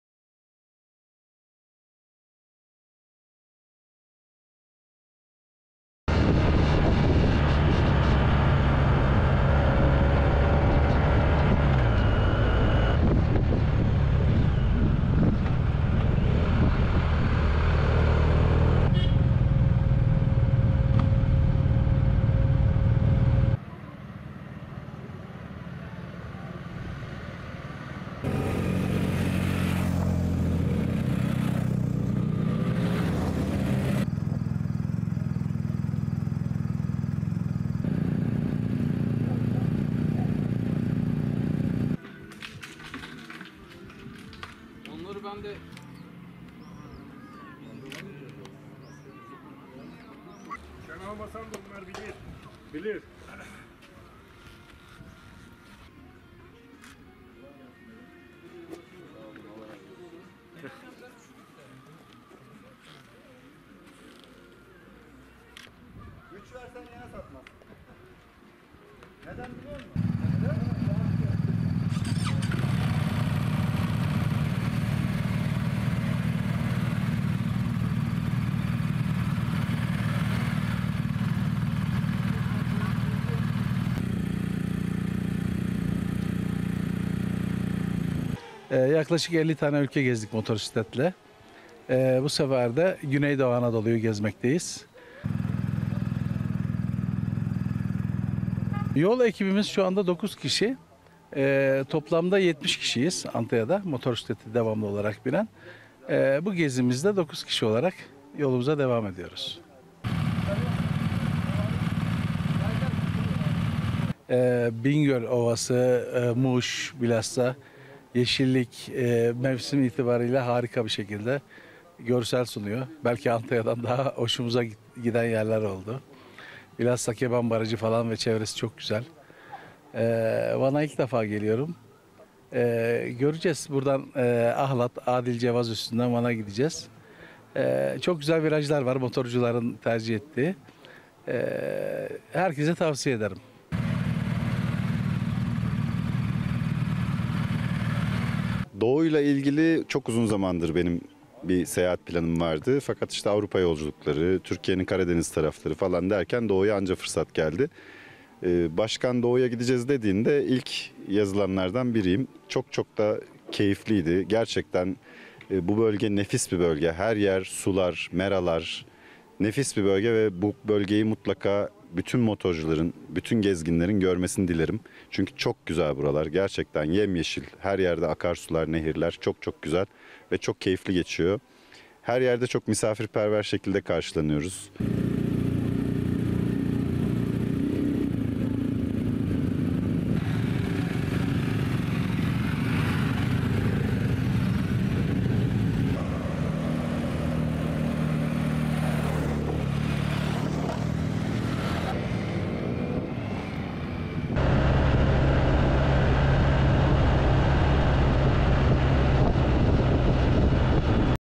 Antalya'dan yola çıkan motosiklet tutkunları Bitlis'e ulaştı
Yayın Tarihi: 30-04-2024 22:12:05